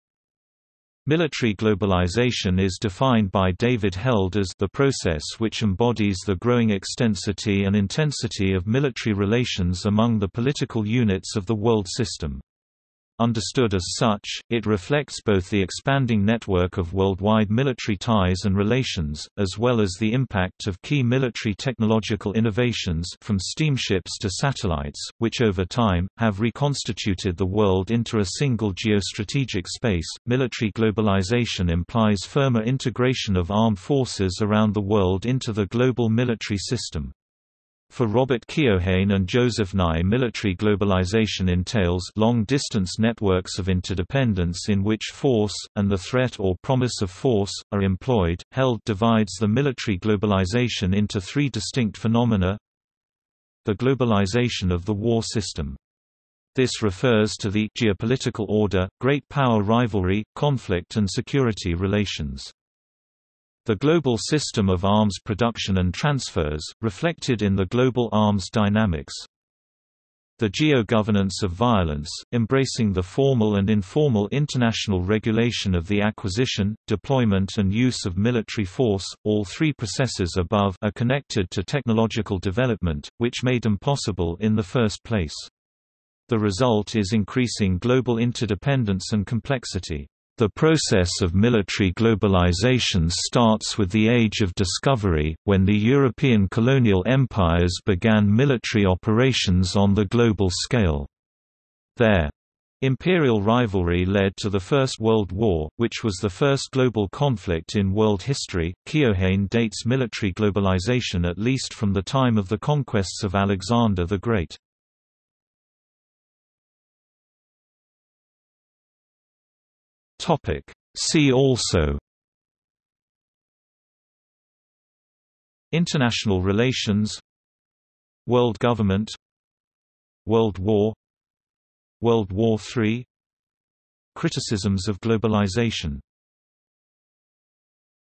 Military globalization | Wikipedia audio article
00:01:56 See also

- Socrates

SUMMARY
Military globalization is defined by David Held as “the process which embodies the growing extensity and intensity of military relations among the political units of the world system. Understood as such, it reflects both the expanding network of worldwide military ties and relations, as well as the impact of key military technological innovations (from steamships to satellites), which over time, have reconstituted the world into a single geostrategic space." Military globalization implies firmer integration of armed forces around the world into the global military system. For Robert Keohane and Joseph Nye military globalization entails “long-distance networks of interdependence in which force, and the threat or promise of force, are employed.”Held divides the military globalization into three distinct phenomena:

The globalization of the war system. This refers to the “geopolitical order, great power rivalry, conflict and security relations.”
The global system of arms production and transfers, reflected in the global arms dynamics.
The geo-governance of violence, “embracing the formal and informal international regulation of the acquisition, deployment and use of military force.”All three processes above “are connected to technological development, which made them possible in the first place. The result is increasing global interdependence and complexity."The process of military globalization starts with the Age of Discovery, when the European colonial empires began military operations on the global scale. Their "imperial rivalry led to the First World War, which was the first global conflict in world history." Keohane dates military globalization at least from the time of the conquests of Alexander the Great.